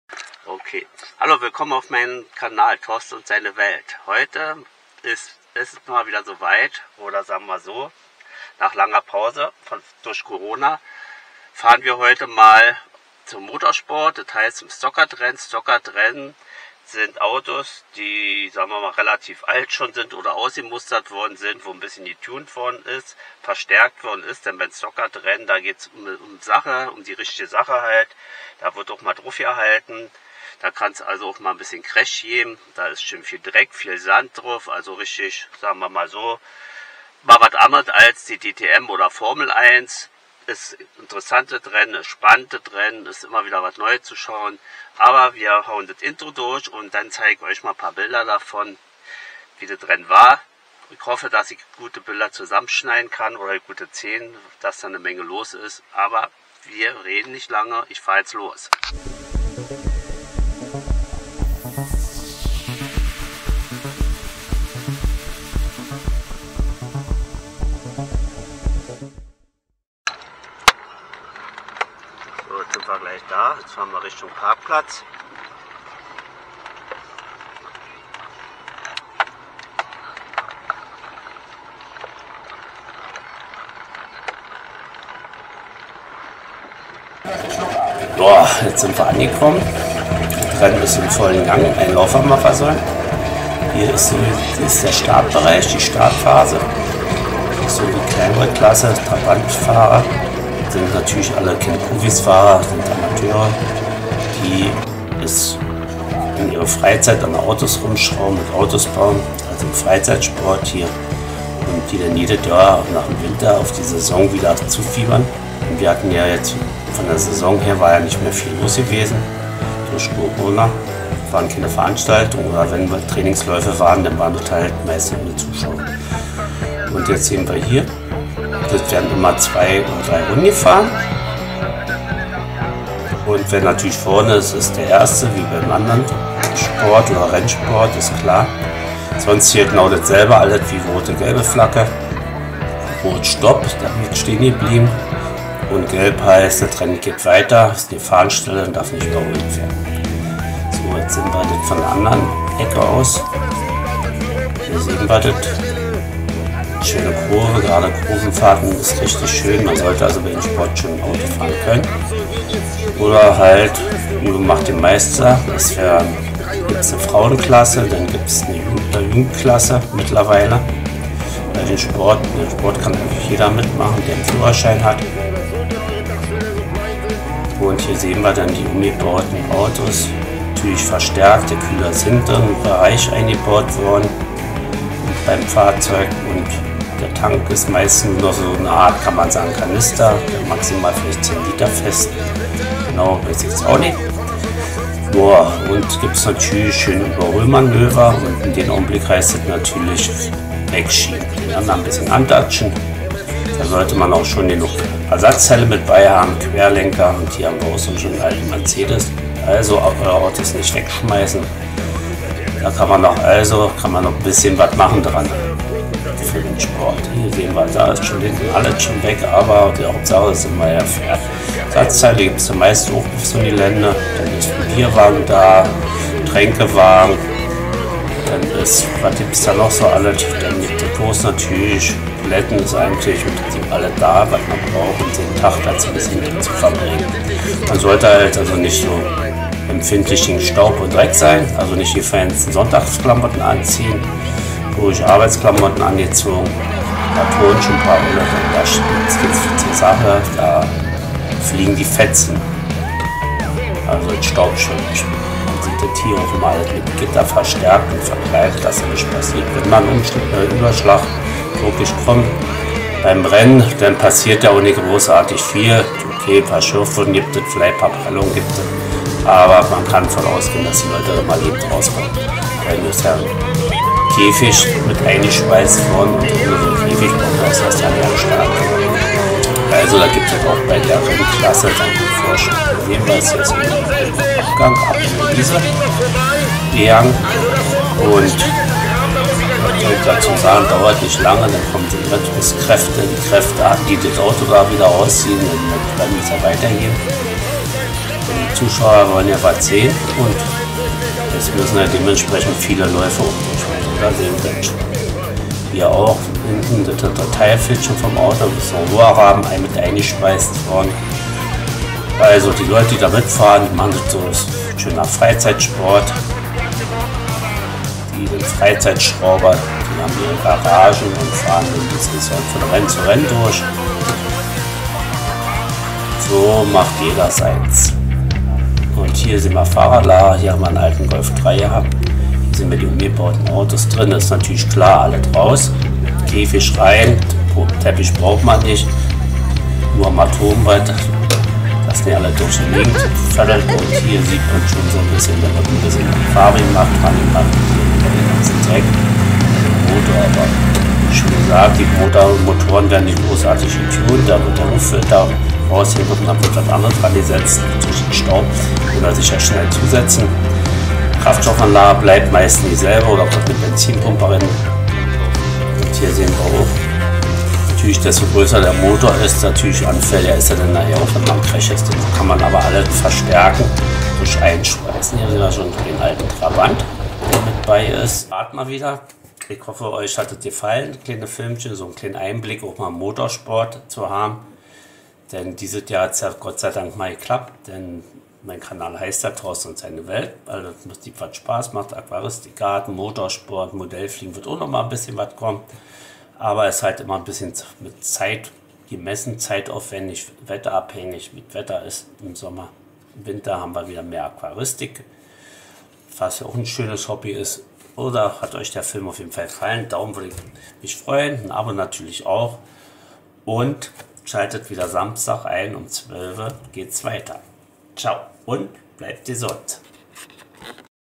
Stockcar - Rennen 31.07.21
Ich zeige heute Euch ein kleines Filmschen vom Motorsport. Es geht um Stockcar-Rennen . Es findet in Brandenburg Altlandsberg  (Stockcar-Arena)